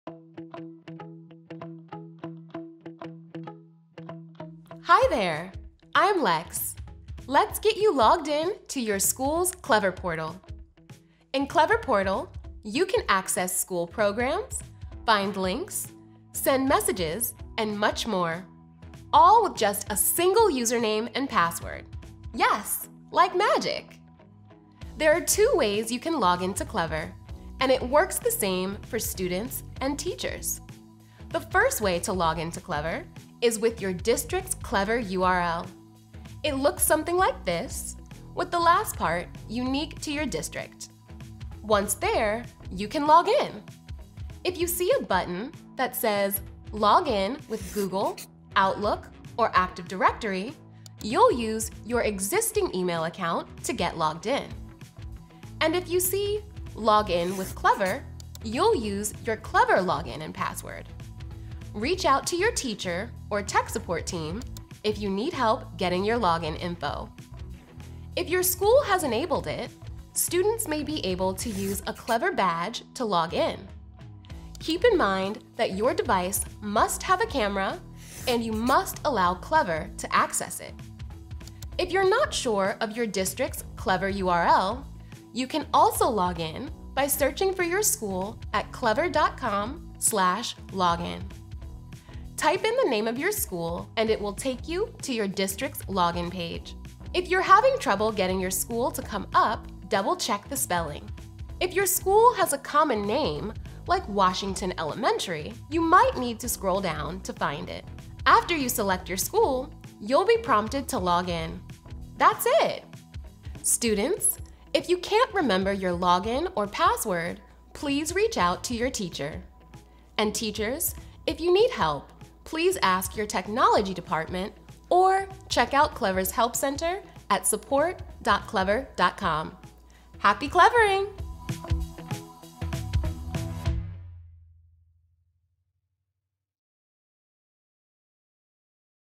Clever Academy: Logging in to Clever
This instructional video explains how students, teachers, and admins can login to the Clever Portal to access all their online learning resources.